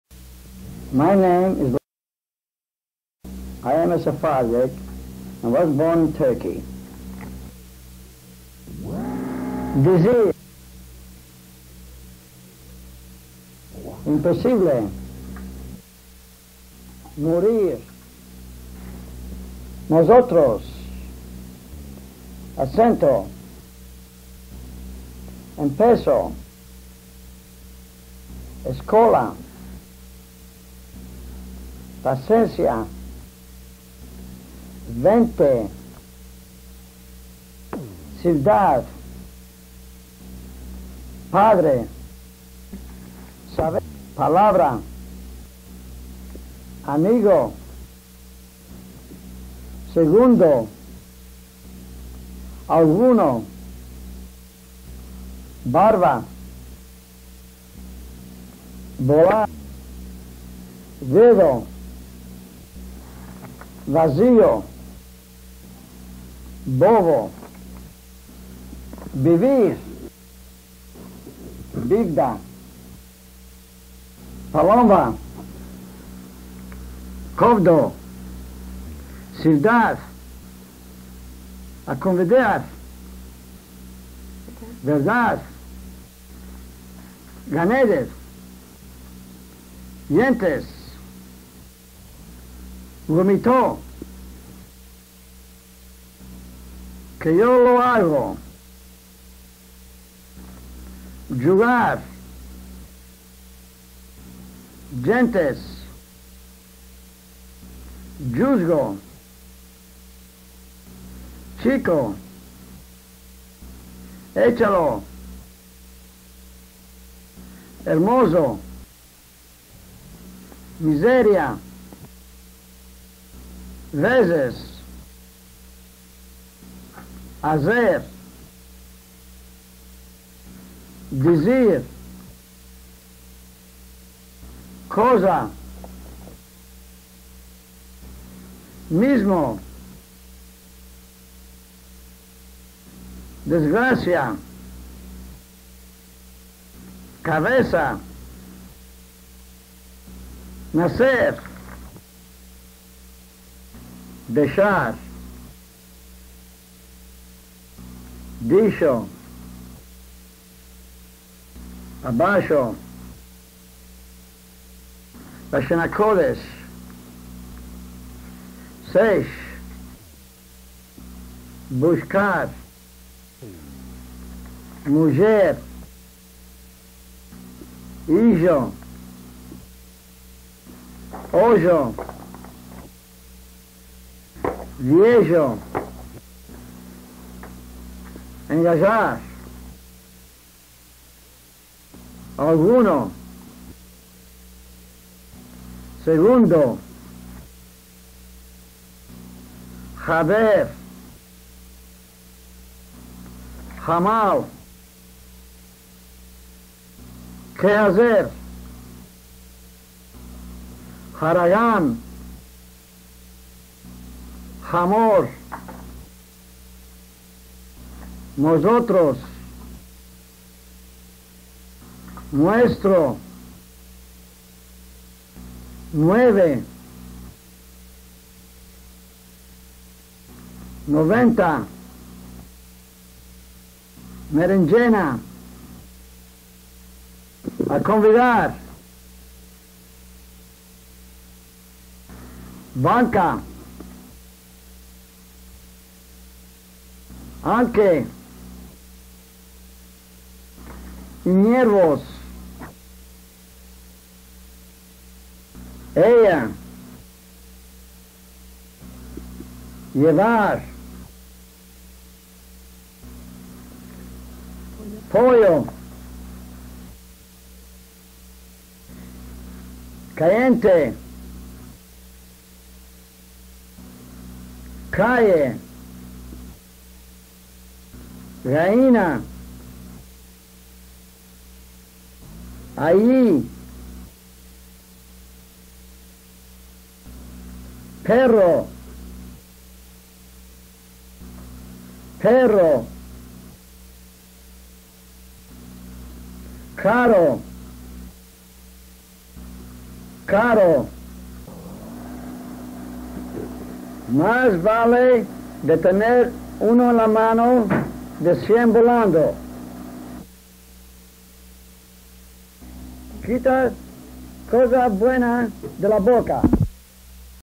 Ladino audio: lad_word-list_1966_01
Ladino (lad) audio: lad_word-list_1966_01
Language family: Indo-European; Middle Indo-European; Late Indo-European; Italic-Venetic; Italic; Latino-Faliscan; Old Latin; Spoken Latin; Hispanic; Gallo-Iberian; Ibero-Romance; West Iberian; Old Spanish;
UCLA Phonetics Lab
Date: 1/14/1966

Description: Language Ladino.  lad_word-list_1966_01.html, enties: 1 - 87

Contributor: N/A
UCLA Phonetics Archive